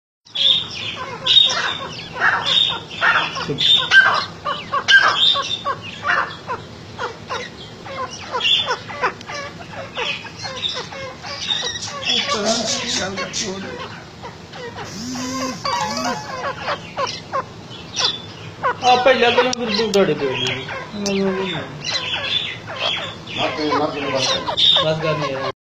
Teetar Fight Partridge Fight in Pakistan
4 Day Old Teetar Babies Dangerous Fight